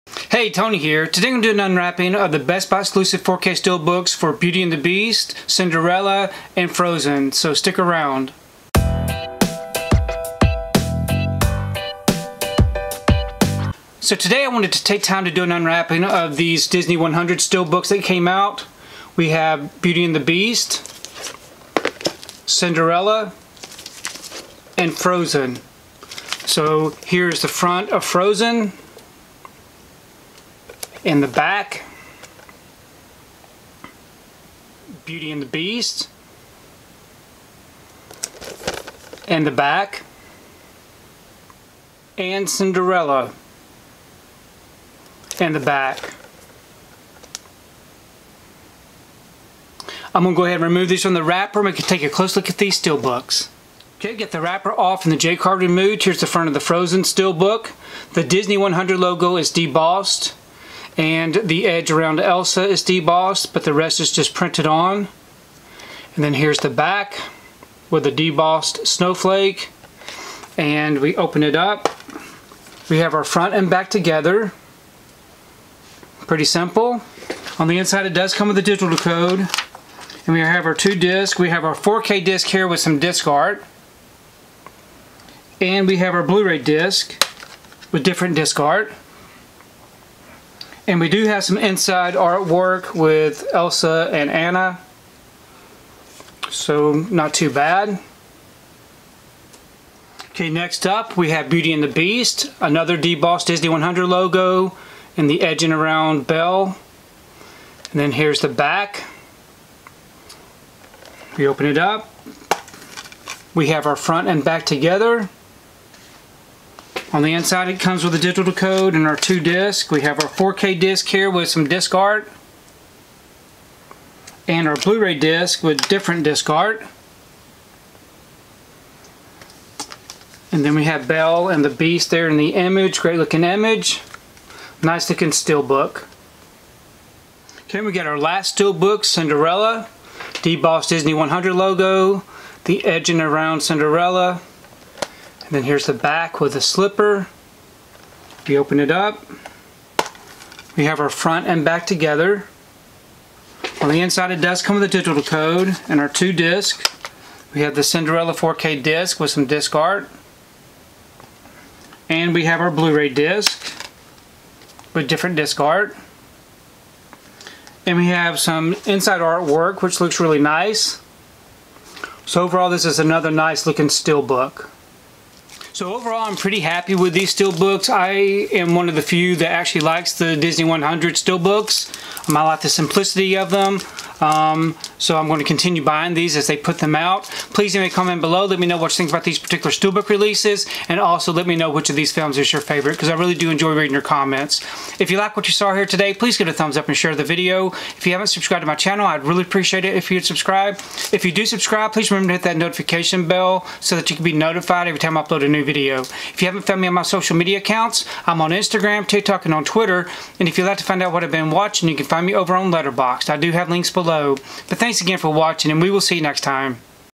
Disney 100 - Disney Princess Best Buy 4K Steelbooks /Frozen /Cinderella /Beauty and the Beast
Check out the Best Buy Exclusive 4K Ultra HD Blu-ray steelbooks for these Disney Princess Films.  Included are Frozen, Cinderella, and Beauty and the Beast!

Frozen
► Purchase the 4K Ultra HD

► Purchase the Blu-ray Combo Pack